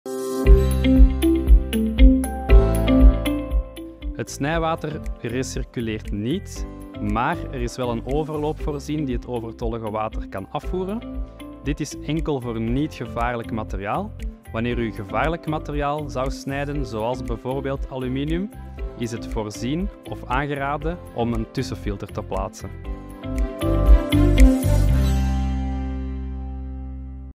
Recirculeert het snijwater bij een Swift-Jet Pro?
Het water dat gebruikt wordt voor het snijden recirculeert niet, maar stroomt weg via de overloopbuis die in de snijtafel is ingebouwd. Deze overloop moet worden aangesloten op een waterafvoerleiding (als u geen gevaarlijk materiaal snijdt). Als u gevaarlijk materiaal snijdt, hebt u een filter nodig.